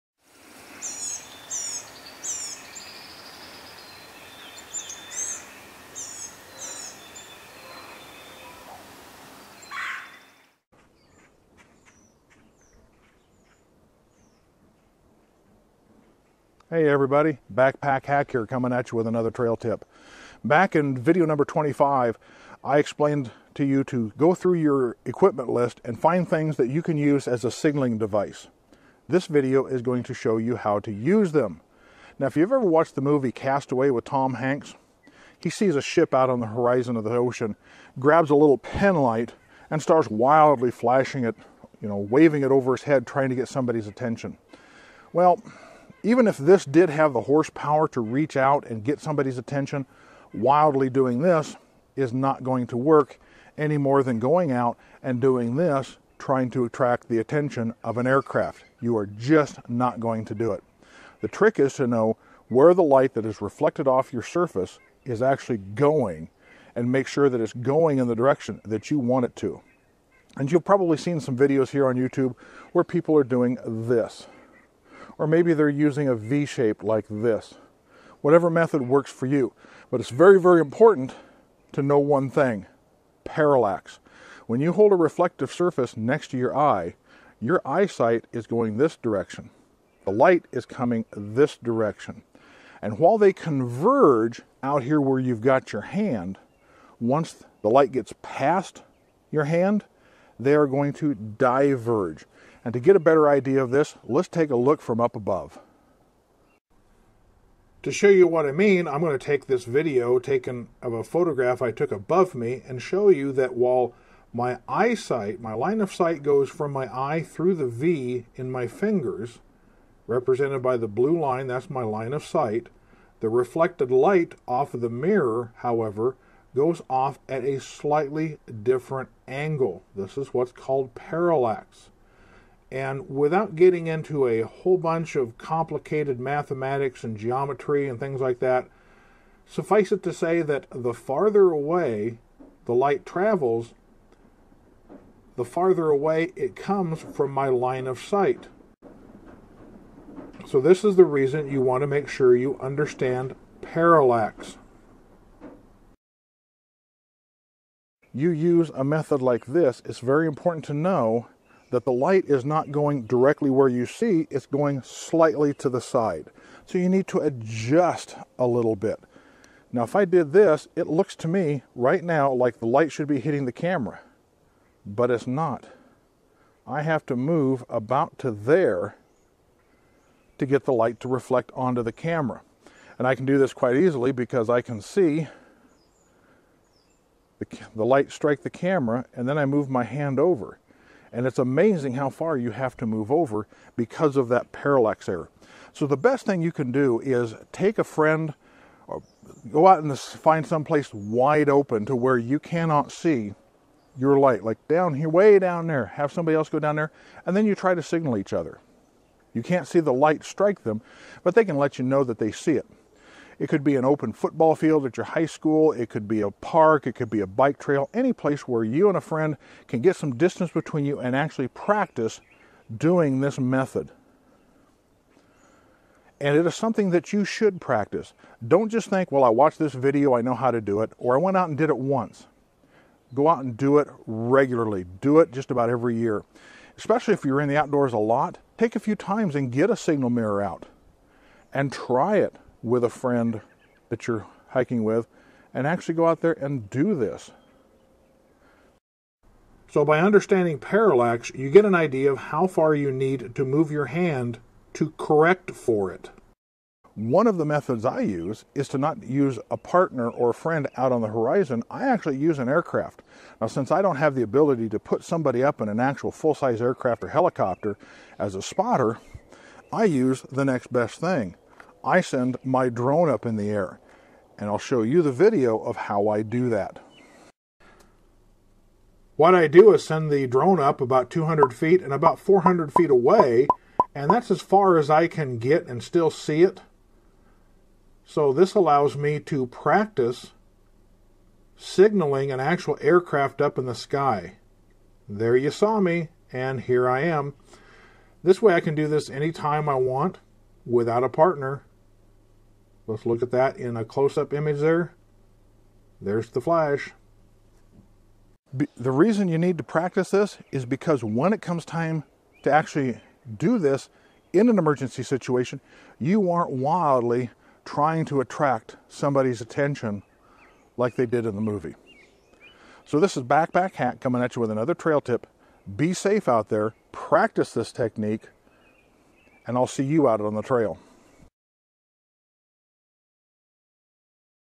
Using a Signal Mirror
Although there are commercially-available signal mirrors available, being able to use any flat, highly-reflective surface to signal rescue personnel (either on the ground or in an aircraft) is a vital and essential skill.

While many YouTube videos make this method appear easy, what many of them fail to demonstrate is parallax.  In this video, I will explain what parallax is, why it's important to understand, and how to compensate for it.

To watch the video (referred to here) about impromptu reflective surfaces one might be able to press into service:

Be safe out there, and I'll see you out on the trail!